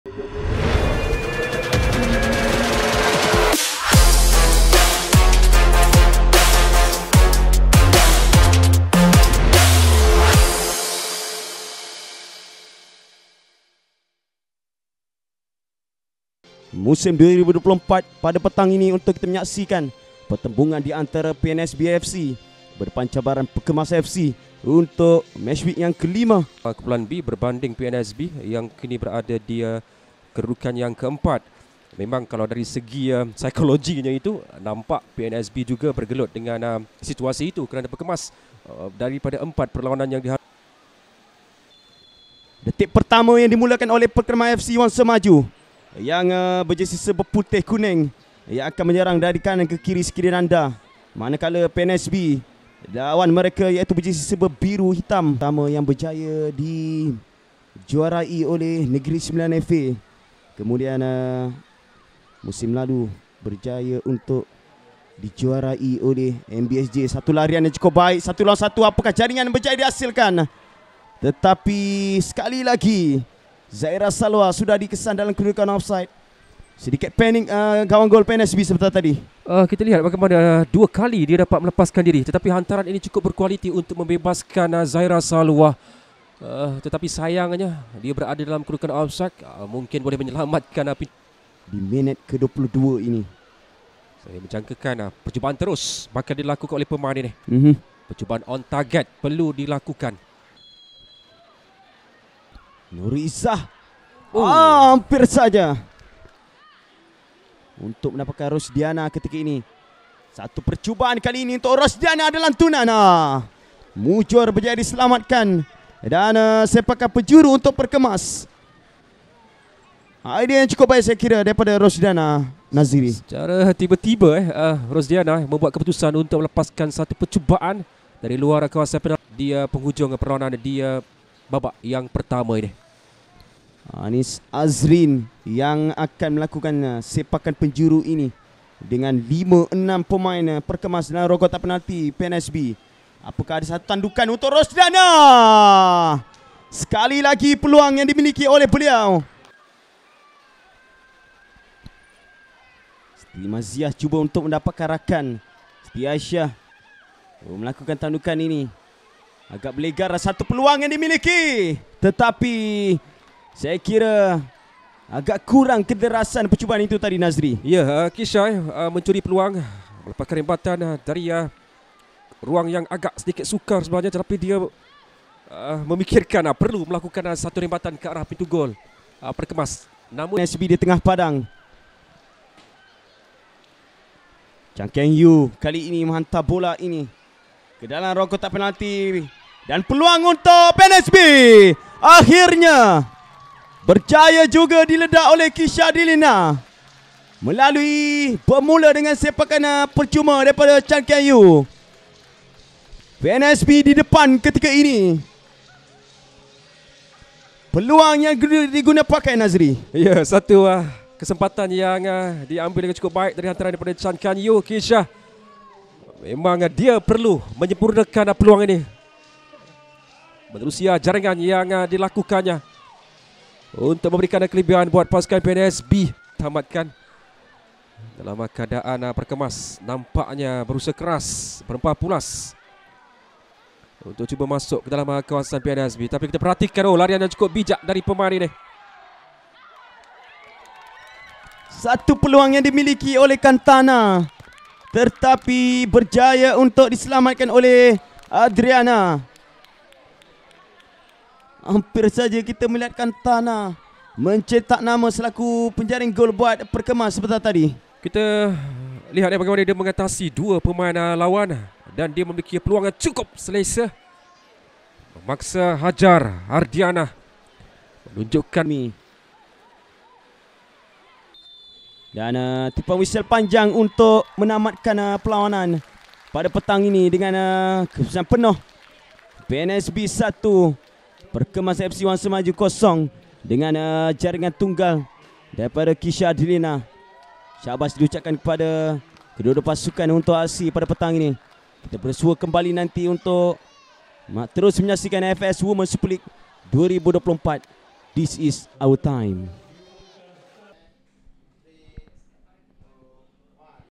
PNSB 1 - 0 PERKEMAS WANGSA MAJU | FAS WSL 2024 Match Highlights
Saksikan sorotan perlawanan PNSB menentang Perkemas di saingan FASWSL 2024.